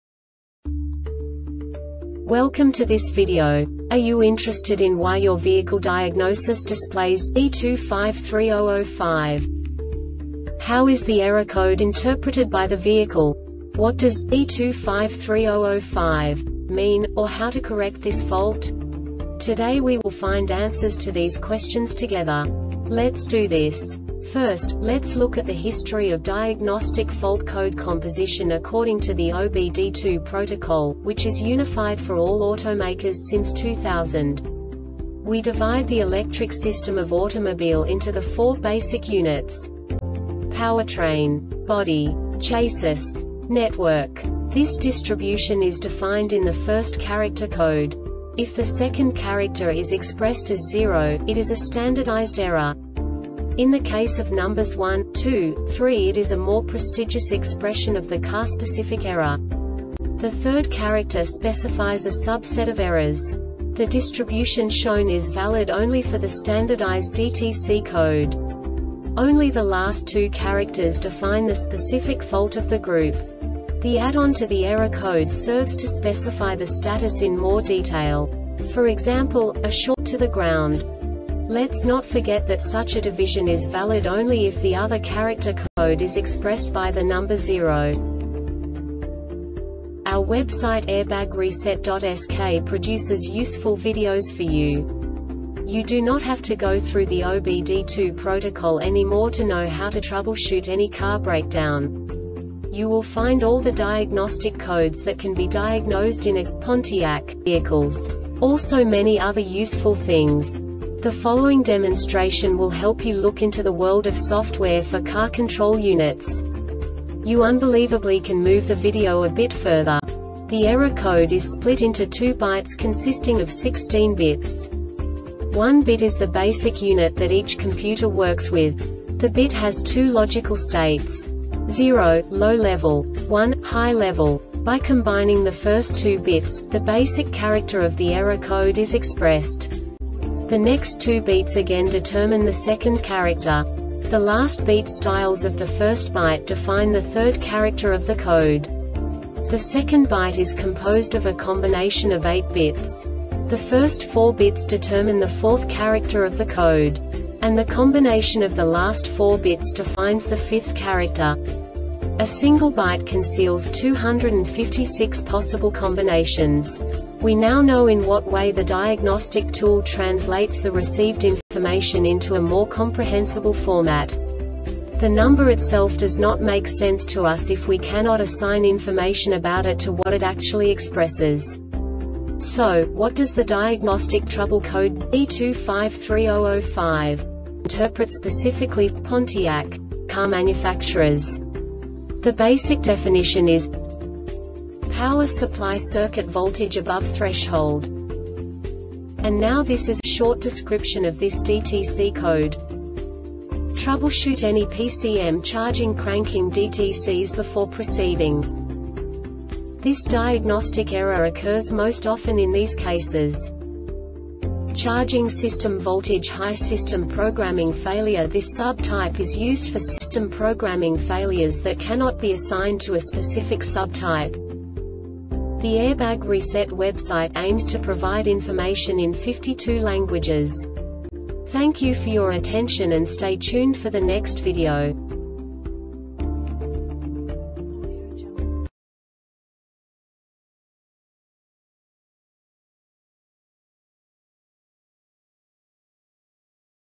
DTC Pontiac B2530-05 Short Explanation
Contents:
0:21 Basic DTC analysis according to OBD2 protocol standard.
1:48 Insight into programming
2:58 A diagnostically understandable description of the Diagnostic Trouble Code
3:05 Basic error definition

Make:
Pontiac
Code:
B2530 05
Definition:
Power-Supply-Circuit-Voltage-Above Threshold
Description:
Troubleshoot any PCM charging or cranking DTCs before proceeding.
Cause:
Charging System Voltage High
Failure Type: